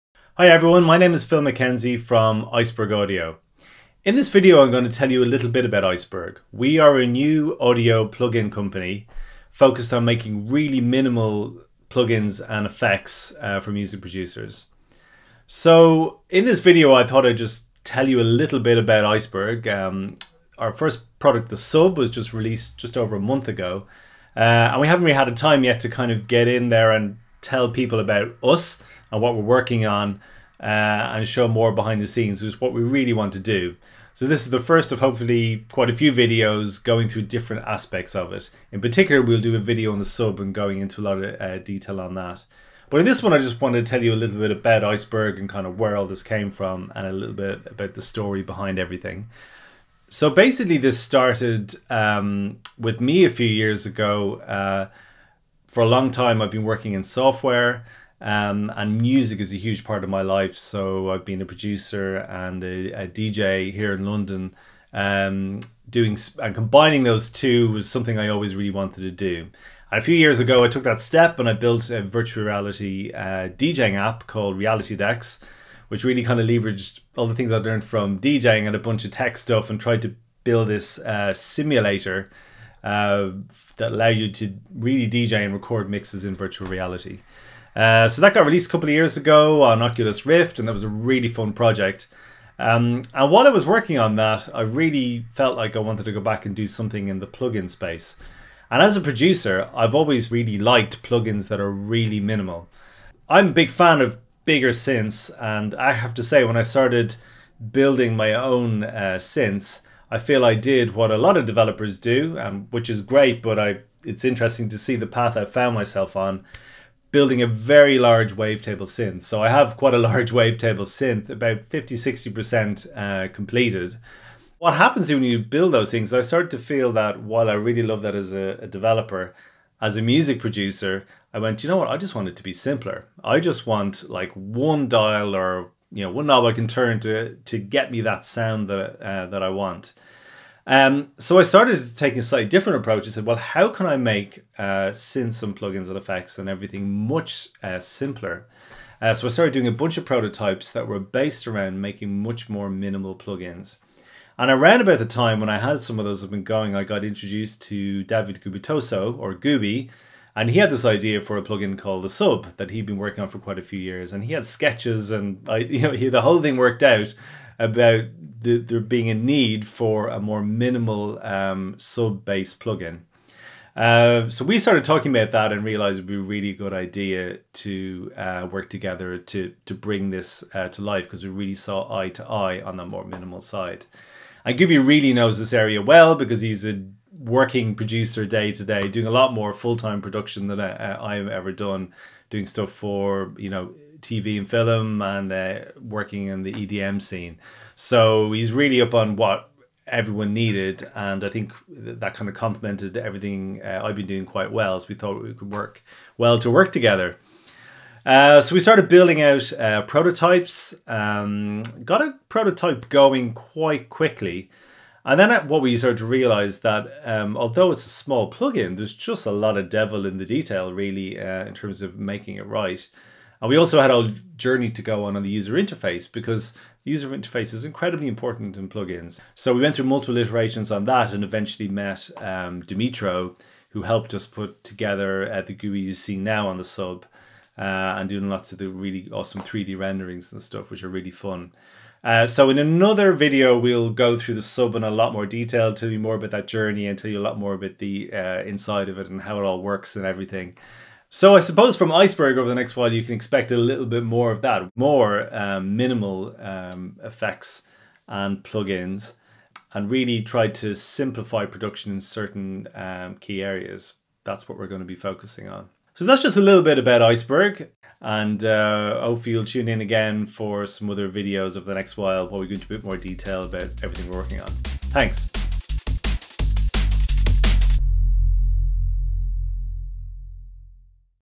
A brief history of Iceberg Audio and developing The Sub
Phil talks about Iceberg Audio, the development of The Sub and building minimal plugins.

The Sub is a minimal sub bass plugin in VST3/AU formats and is available on a 14 day trial from:

by Davide Gubitoso and Philip Mackenzie